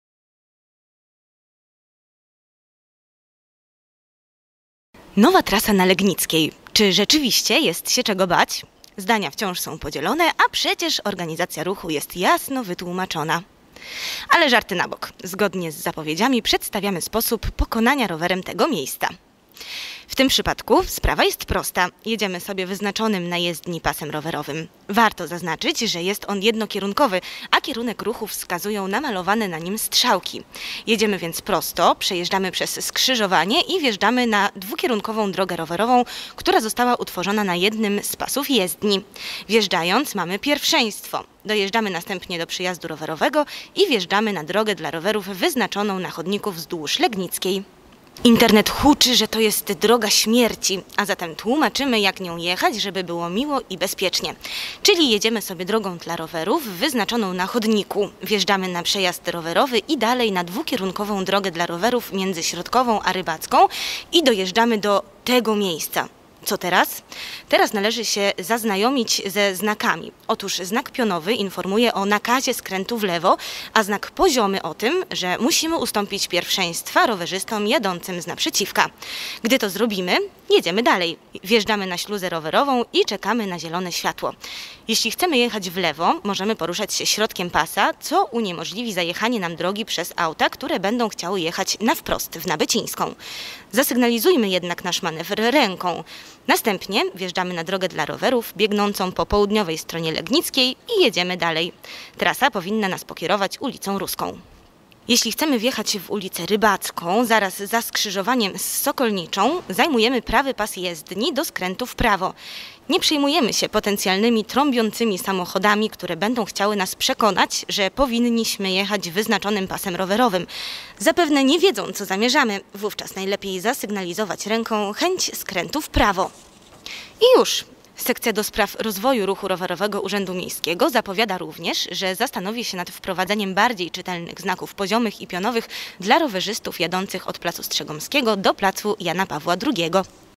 Jak korzystać z trasy rowerowej na ul. Legnickiej?
JAK JEŹDZIĆ NOWĄ TRASĄ ROWEROWĄ NA LEGNICKIEJ❔❔❔
📢 Zgodnie z zapowiedziami przedstawiamy kolejny odcinek Wrocławskiego Wideoporadnika Rowerowego. Tym razem tłumaczymy jak korzystać z nowej trasy rowerowej na Legnickiej (szczególnie na odcinku między Rybacką a Środkową) 💡🚲
🎬 Od razu dodajmy, że po nakręceniu filmu skontaktowaliśmy się z Sekcją ds. Rozwoju Ruchu Rowerowego Urzędu Miejskiego Wroclaw [Wroclove] i dowiedzieliśmy się, że dla osób jadących od pl. Strzegomskiego do Jana Pawła II pojawią się sierżanty rowerowe na skrzyżowaniu między Rybacką a Nabycińską